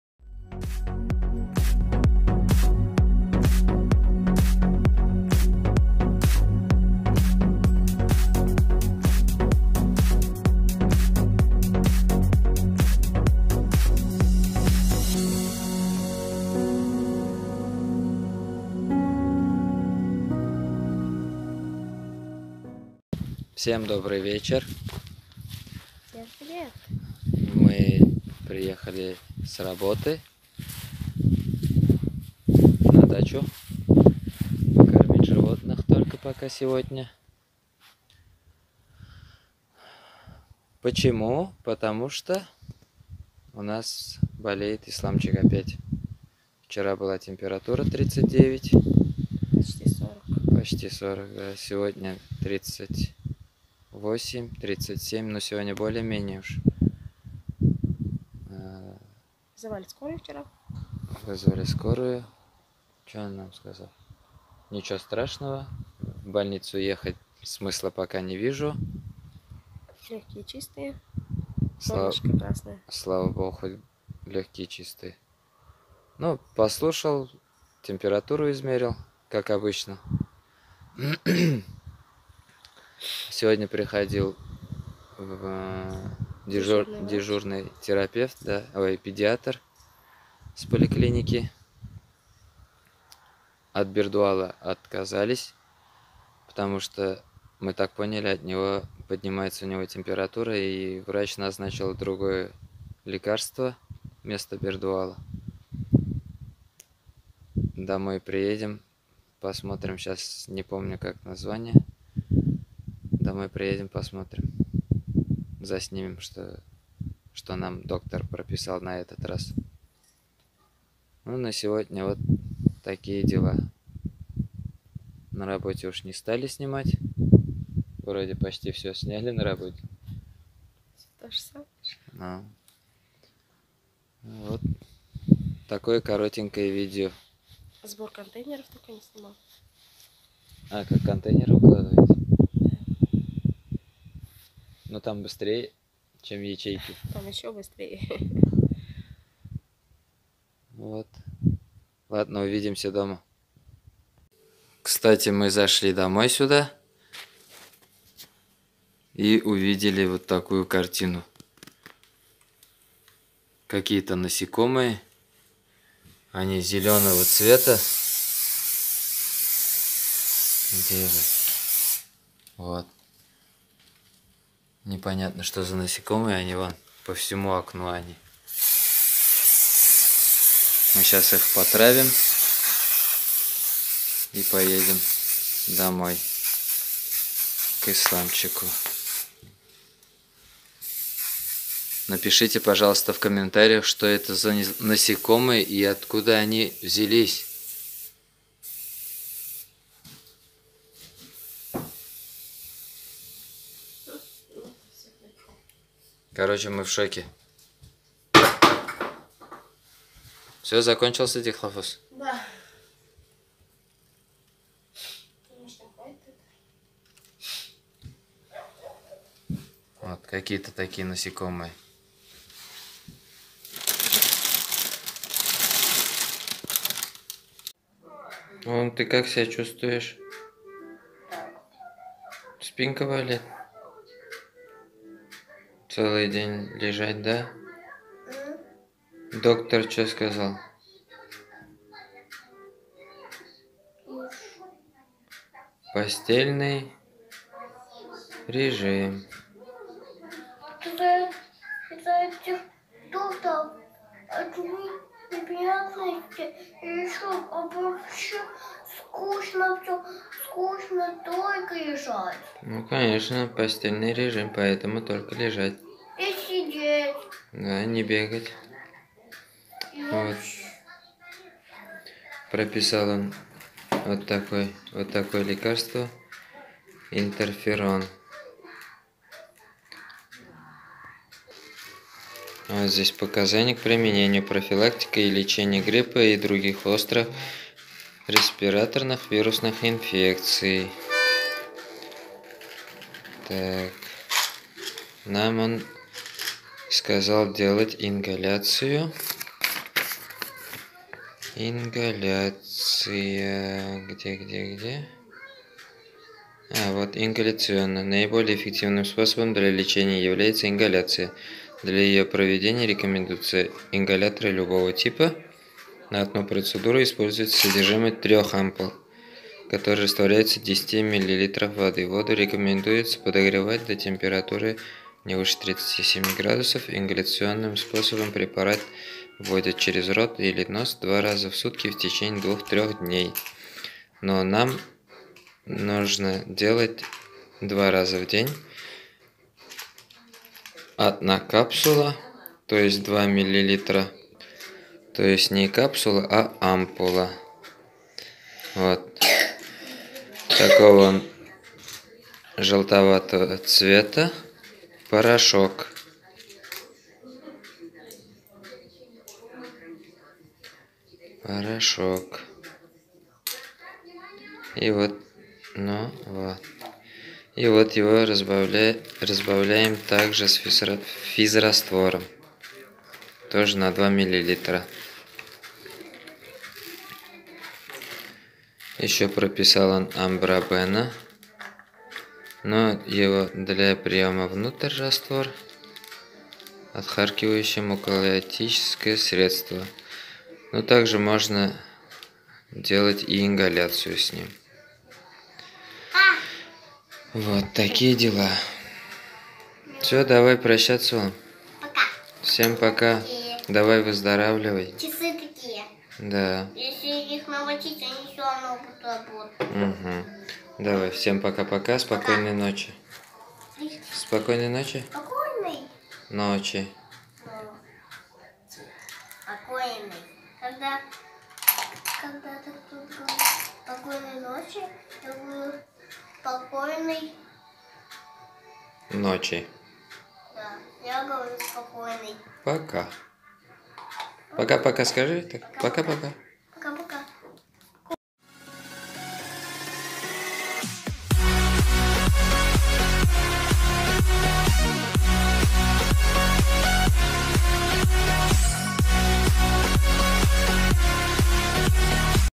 У сына высокая температура. Назначили другой препарат//04.10.2019
Вчера у Нашего сына, поднялась температура, почти 40° . Еле сбили до 38. с помощью растирания спины водкой.
Слава Богу, ему стало легче и он уснул.
А сегодня пришёл дежурный педиатр из поликлиники и прописал другие лекарства.
А какие ,смотрите видео!!!
Всем Спасибо за поддержку Друзья!

💥 Наши Плейлисты 💥